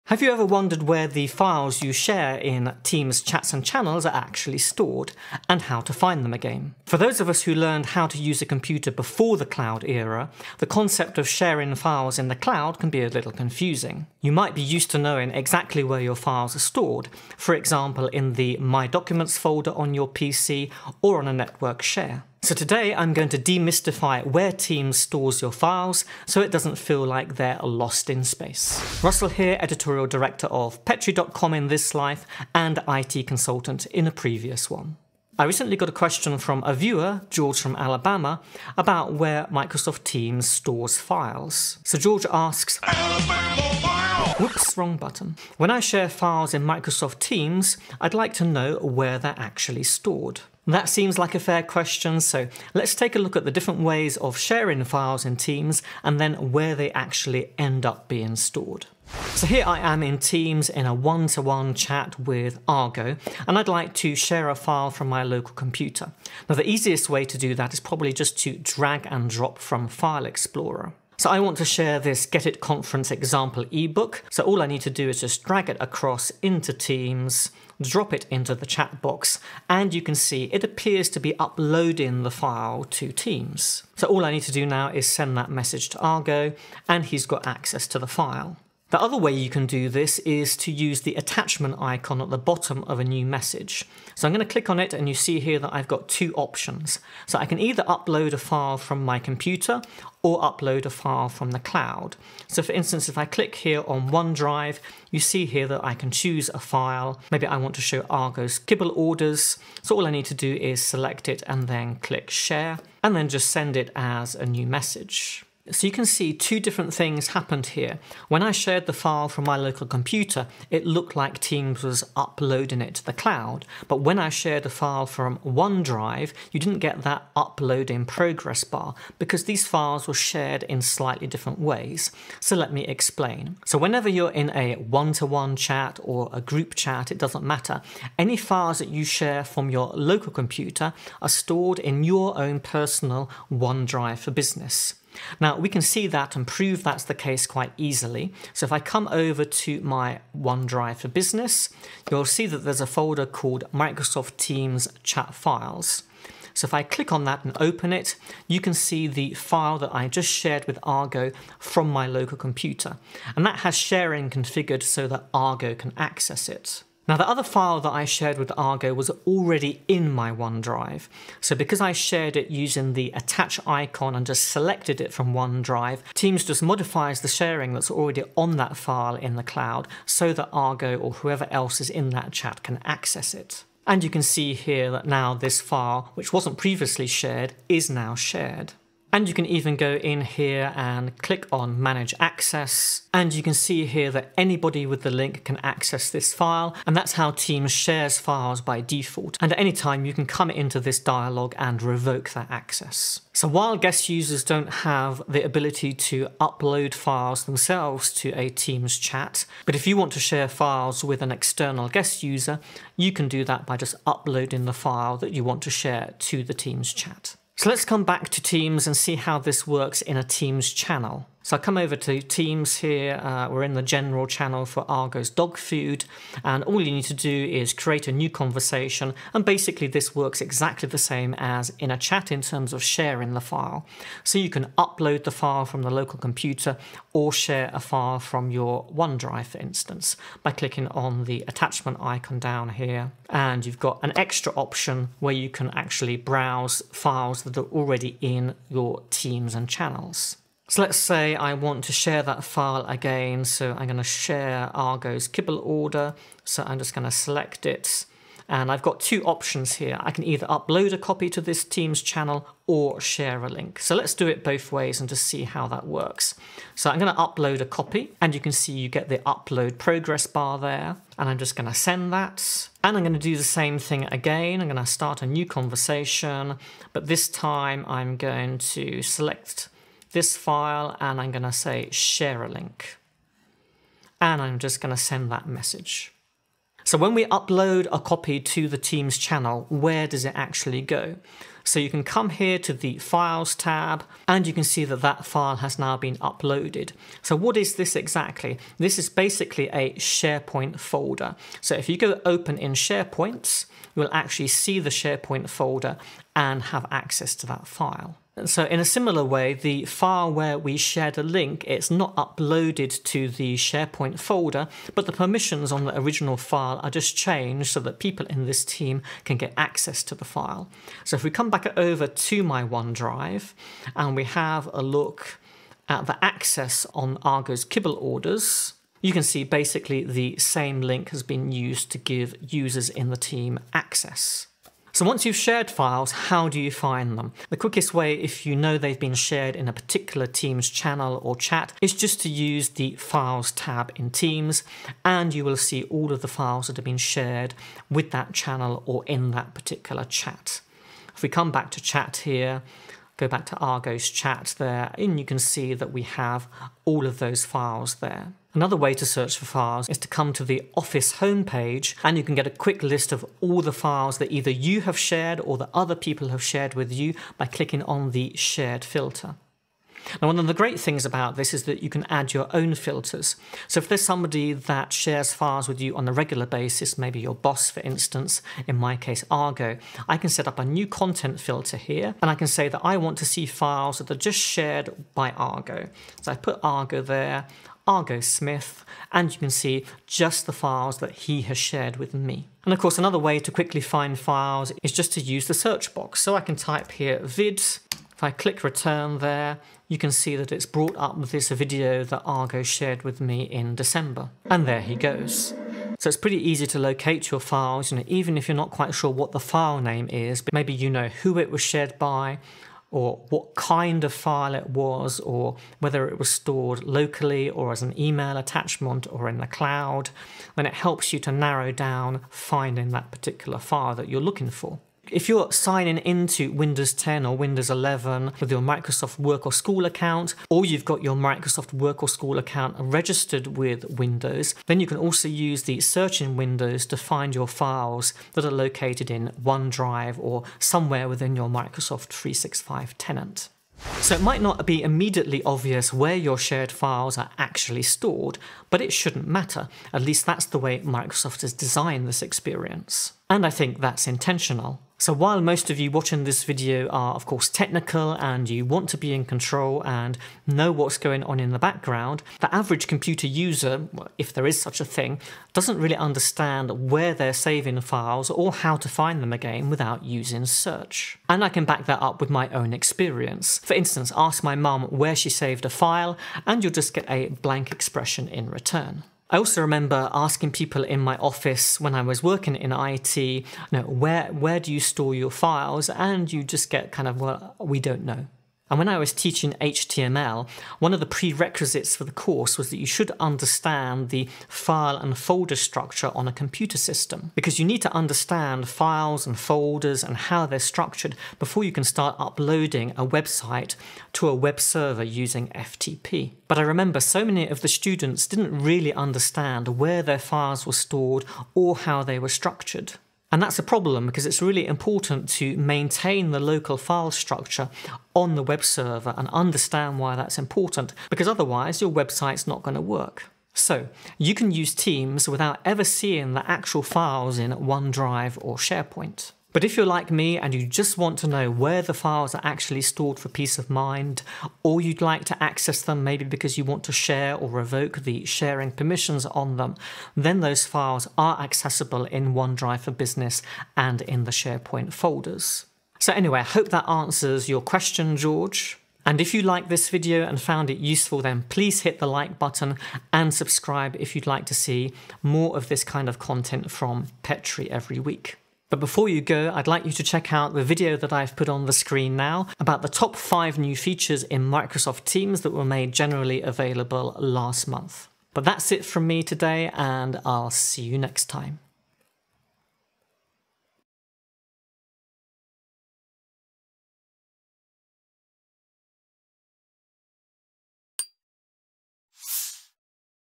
Where Does Microsoft Teams Store the Files I Share?
This week in IT, I explain where Microsoft stores your shared files, the different ways to share files, and where you can view and revoke sharing permissions.